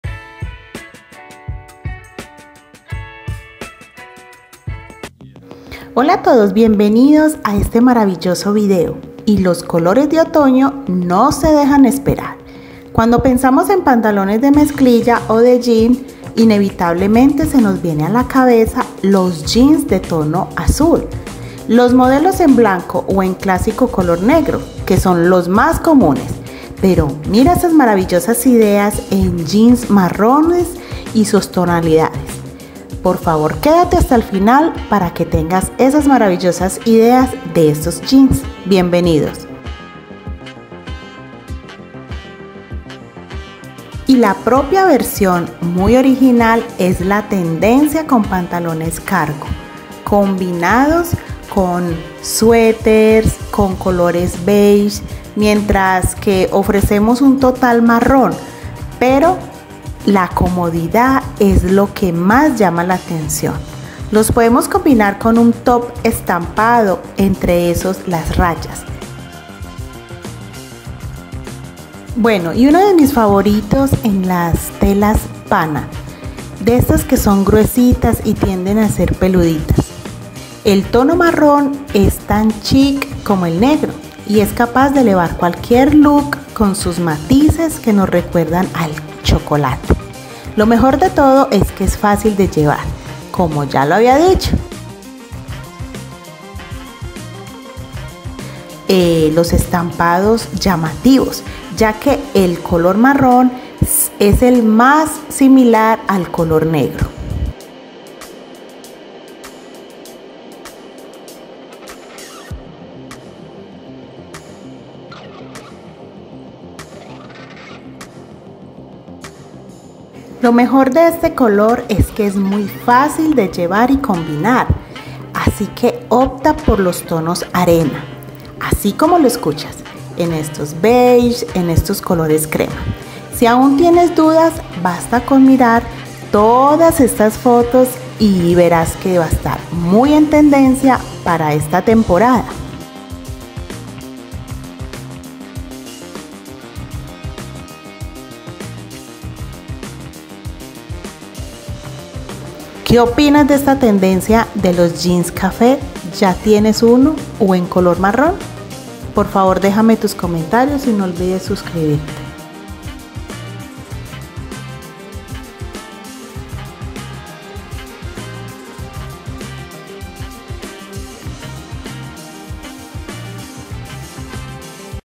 LLEGO EL MARRON PARA ESTA TEMPORADA
Recordemos que el tono marrón es tan chic como el negro, y es capaz de elevar cualquier look con sus matices que nos recuerdan el chocolate.